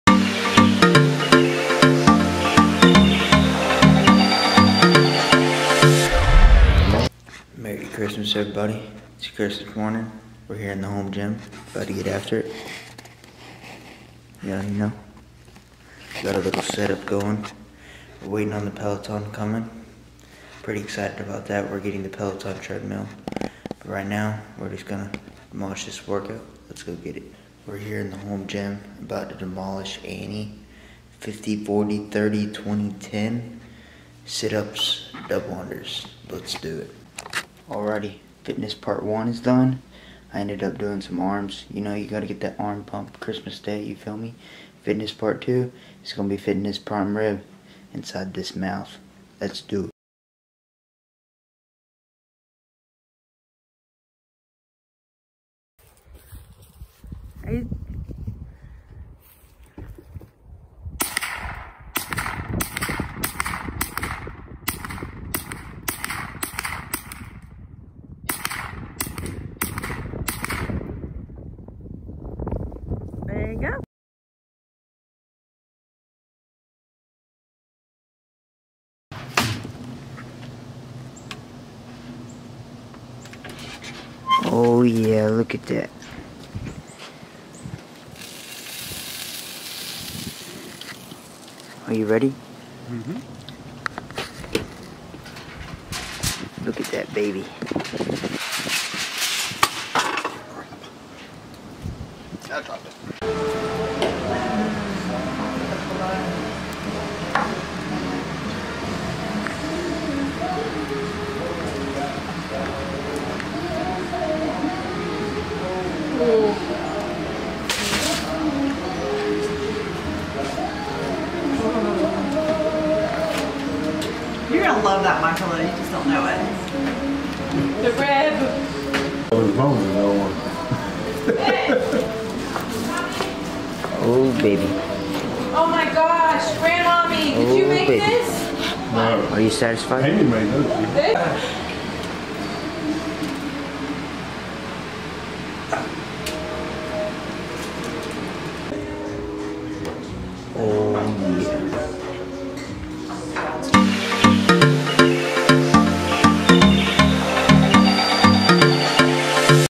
Cooking a tomahawk prime rib!!!
Tomahawk Prime Rib Christmas Dinner! Paired with a great CrossFit workout, shooting, normal blood sugars, cauliflower mash, and brussel sprouts!